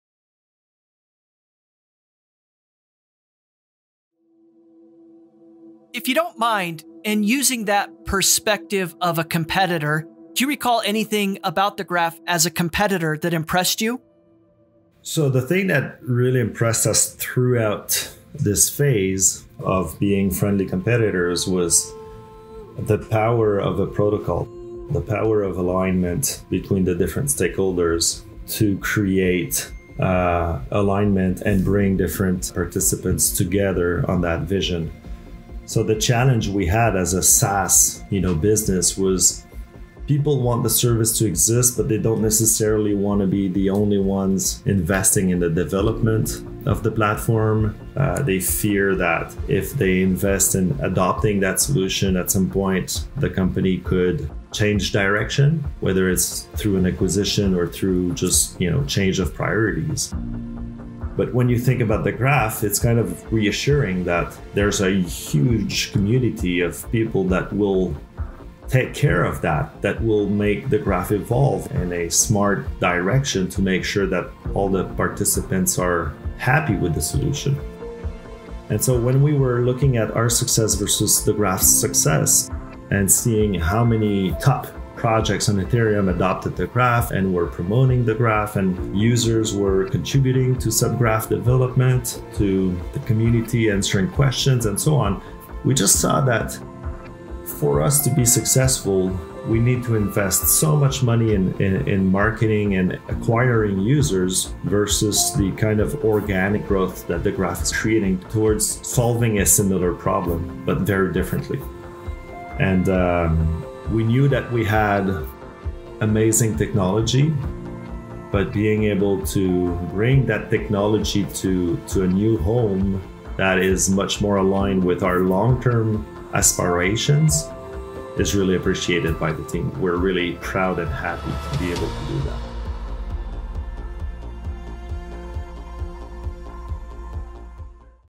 Marc-Antoine Ross Discusses Moving from Competitor to Partner with The Graph
Episode 15: Today’s podcast is a special release – I’m speaking with Marc-Antoine Ross, the CEO and a Co-founder of StreamingFast, a protocol infrastructure company.

On June 17, 2021, The Graph and StreamingFast announced a new partnership, which has since received a lot of news and industry attention. Due to the nature of this new partnership, and the impact it will have in The Graph community, I wanted to record a conversation with Marc and make it available as quickly possible.

My conversation with Marc is incredible! In addition to sharing his professional background and entry into crypto, Marc discusses his life-long passion for entrepreneurship, insights into the origins of StreamingFast, the problem he and his team initially set out to address, and what this new partnership will mean for the future of The Graph.